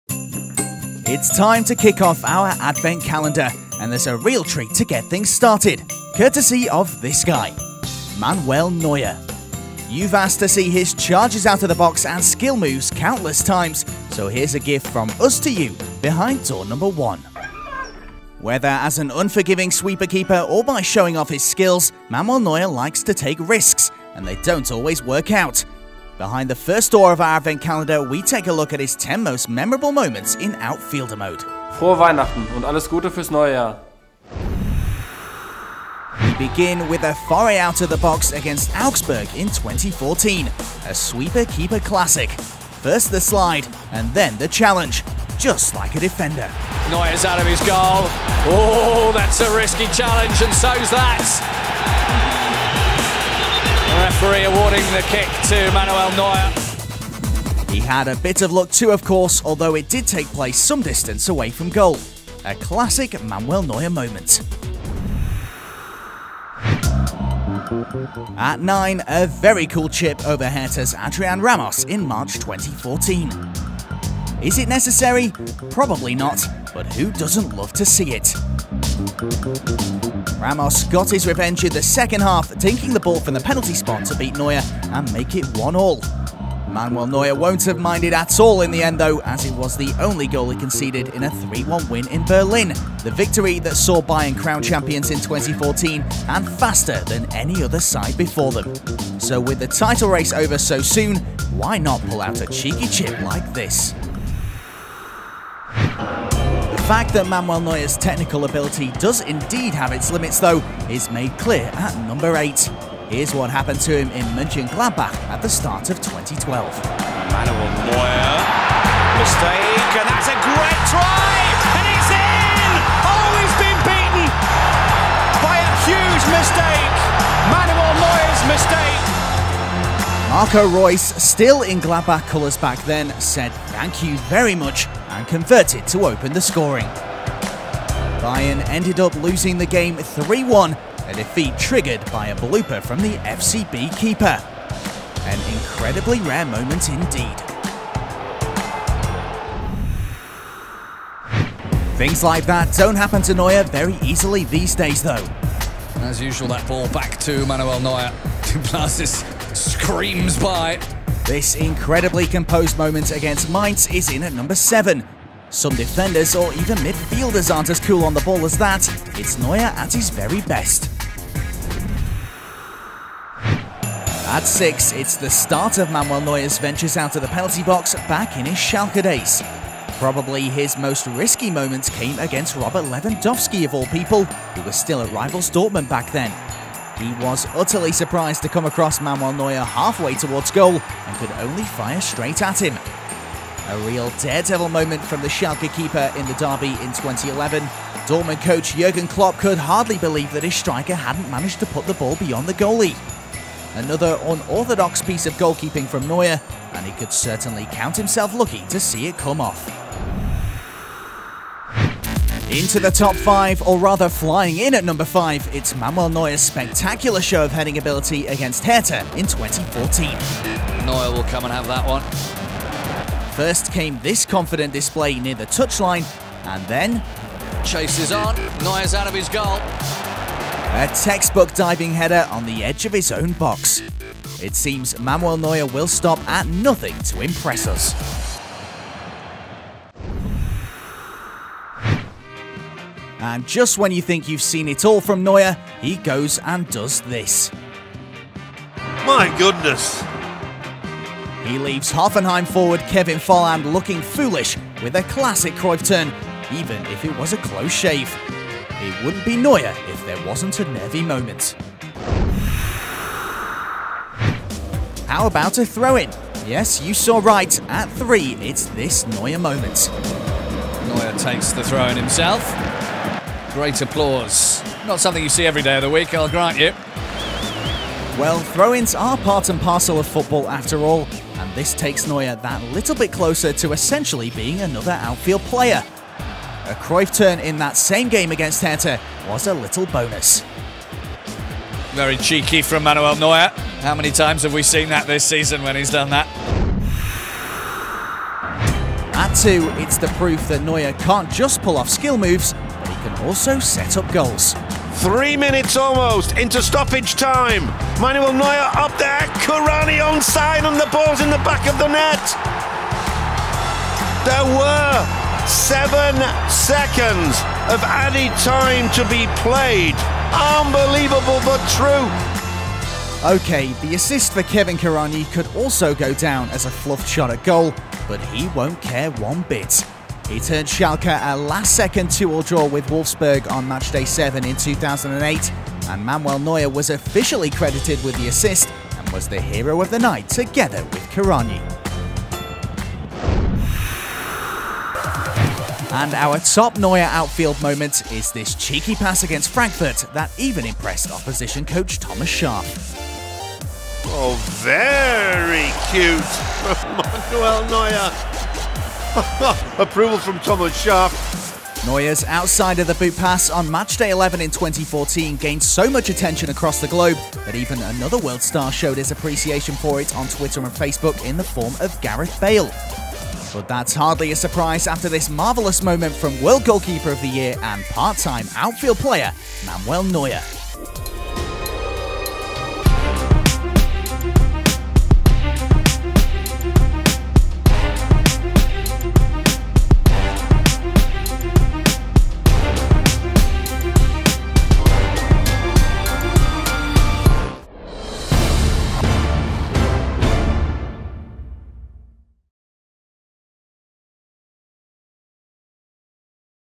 Top 10 Manuel Neuer Sweeper-Keeper Moments - Advent Calendar 2015 Number 1
Today is 1st December, meaning it’s time to open the first door on those Advent calendars, and the Bundesliga YouTube channel is no exception! We’ve got a real highlight for you to kick things off this year. Loads of you asked to see Manuel Neuer simply being Manuel Neuer, so here are his top 10 moments as an outfield player for you to enjoy.